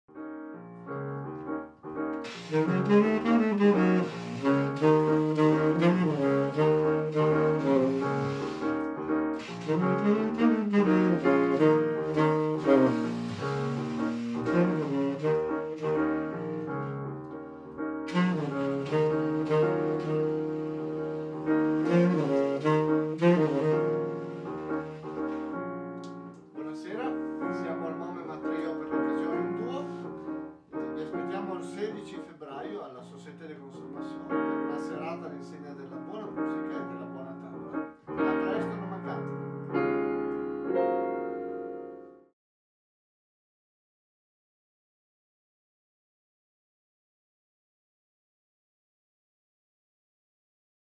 Cena-concerto del MoMeMa Duo Jazz al Ristorante Société En Ville di Aosta
Video promo per la serata evento di sabato 16 febbraio 2019 presso il ristorante "Société en Ville" di Aosta.